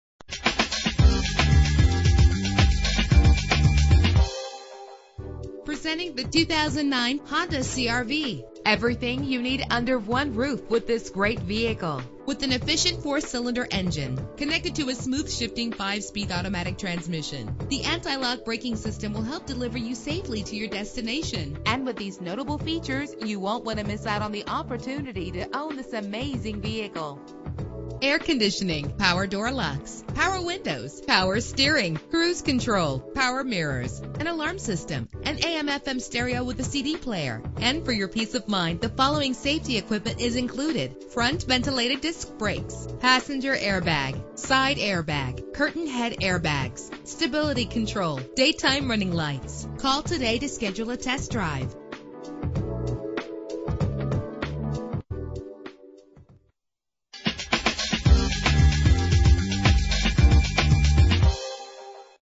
2009 Honda CR-V - Denison TX
Year: 2009
Make: Honda
Model: CR-V
Trim: LX
Engine: 2.4 liter inline 4 cylinder 16 valve
Transmission: 5-Speed Automatic
Color: Gray
Mileage: 29752
Address:
2020 U.S. Hwy 75 N.